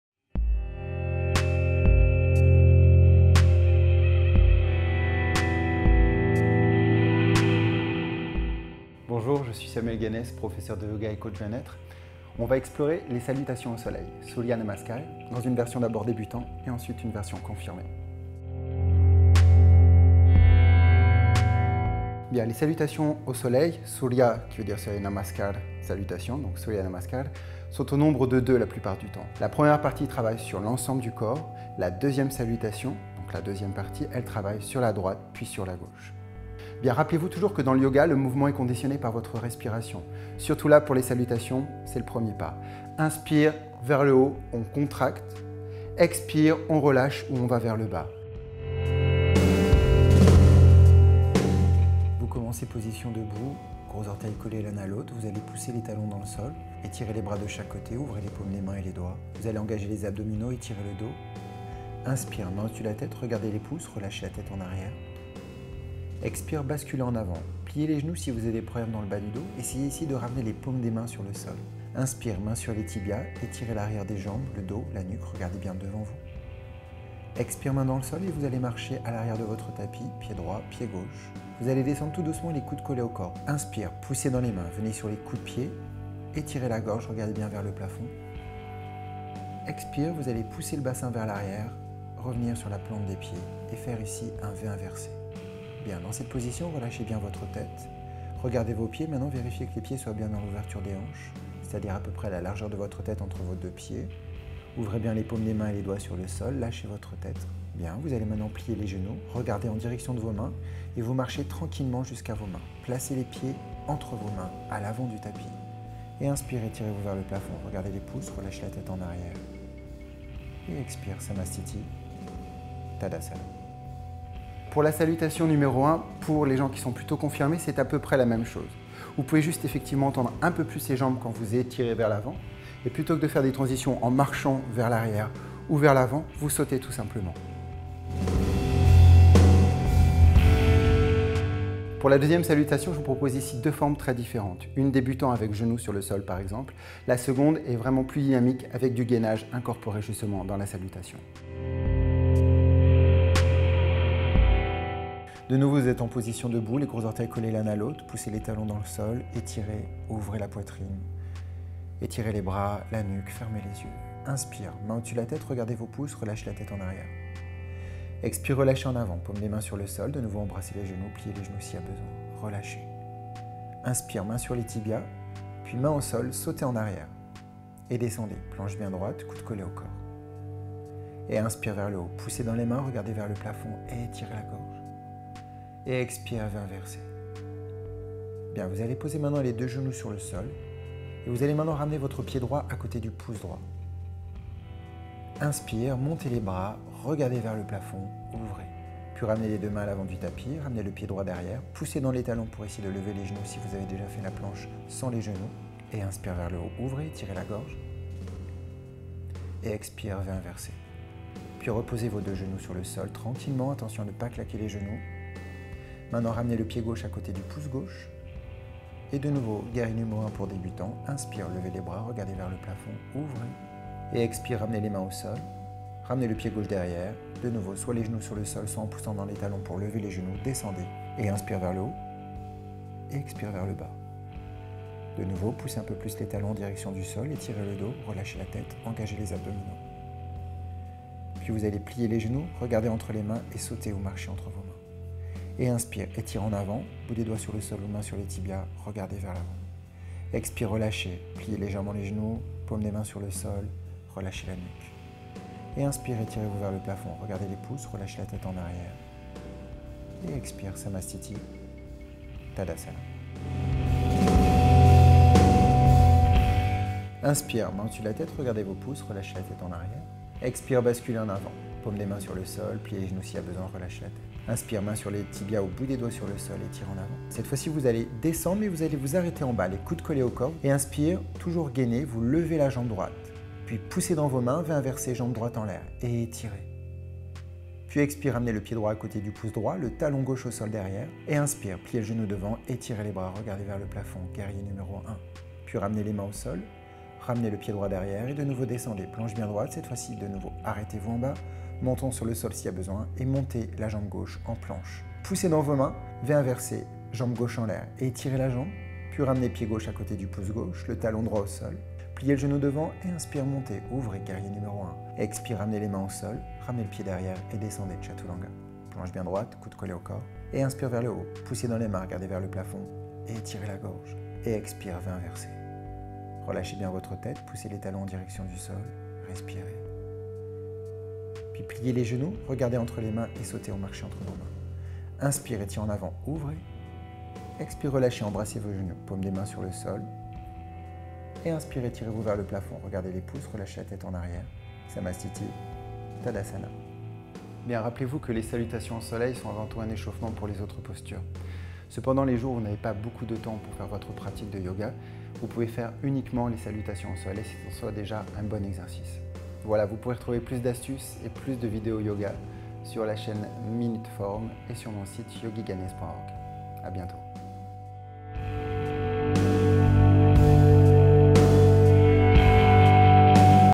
Postures de yoga Salutation au soleil avec un prof
Initialement on prescrivait d'effectuer une Salutation au Soleil tous les matins mais également chaque soir, de préférence face au Soleil. Samuel Ganes, coach bien être vous en dit plus sur cette tradition que l'on appelle également SURYANAMASKAR.

En réalité il s'agit est une série de 12 mouvements synchronisés avec le souffle qui permettent de préparer le corps à l'exécution des postures.

Cet exercice permet à lui seul de maintenir un important degré de souplesse et de tonus musculaire

Lubrifie les articulations, réduisant le risque de blessure à ce niveau. C'est un des bienfaits principaux. C'est à cela que sert la salutation au soleil.

Notez que la salutation au soleil est adaptée à la grossesse (femme enceinte de 0 à 6 mois).

Pratiquez le yoga avec un coach. Suivez sa formation et ensuite vous pourrez effectuer les postures à la maison devant un DVD par exemple. Pratiquez un échauffement soutenu.

Dans la même série vous avez également la salutation face lune

N'hésitez pas à donner votre avis dans les commentaires !

Tutos forme, santé, bien-être... Retrouvez tous nos conseils forme en vidéo

Remerciements :
Samuel Ganes